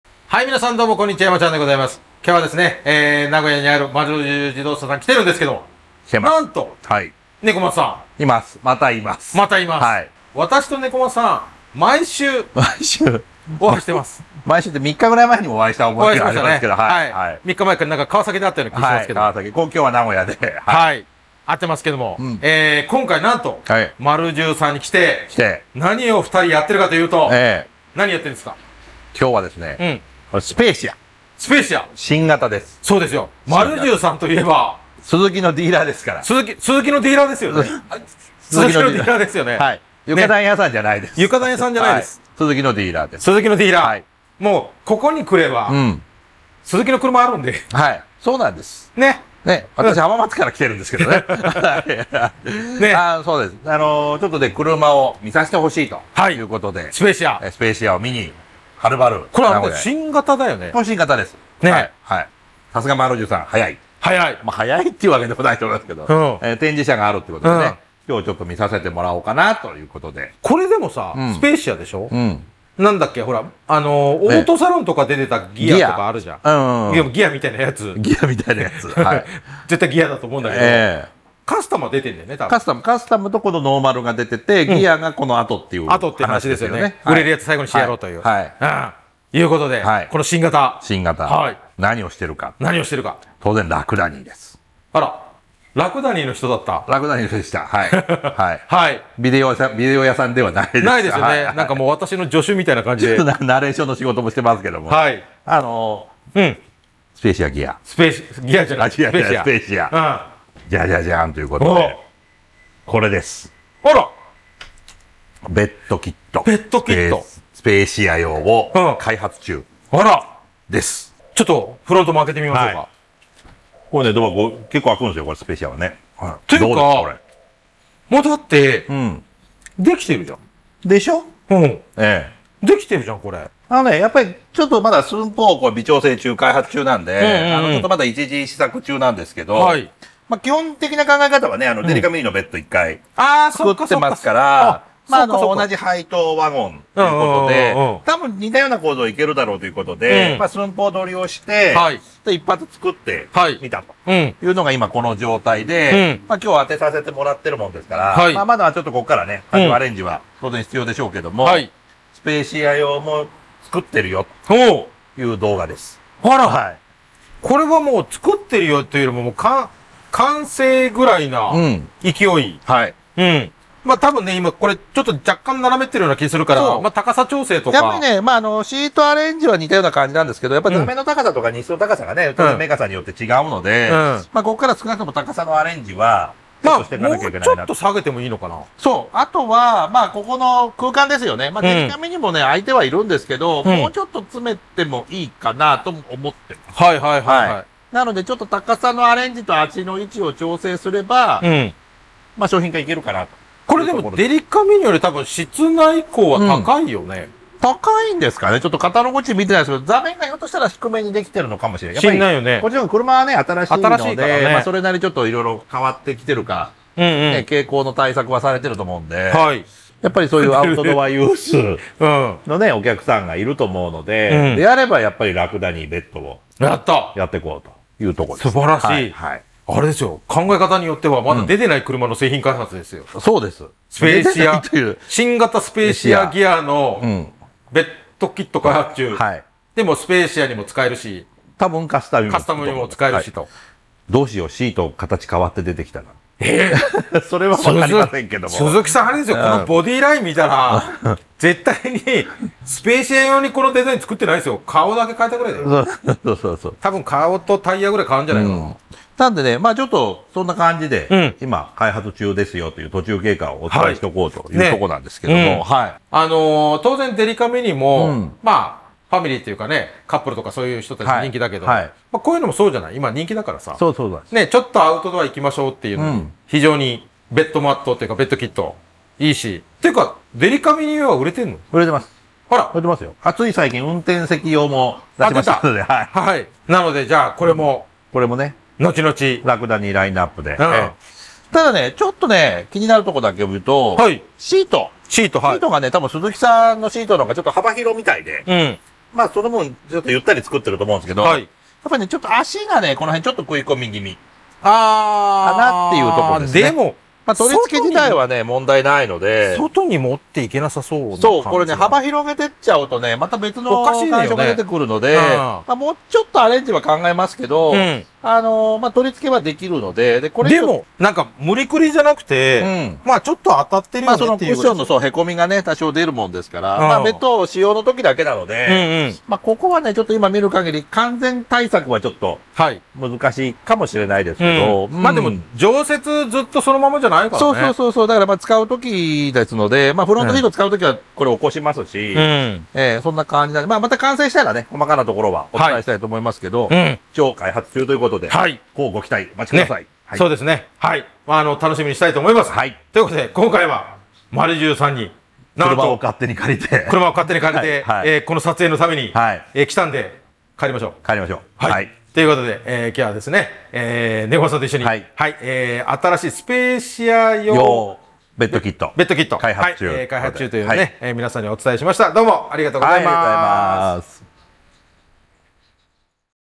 新型スペーシア用ベッドキット開発中！
■CRAFT AOIのYahoo!ショッピング

デリカミニで大人気のベッドキット。新しく新型スペーシア用ベッドキット開発中なのです。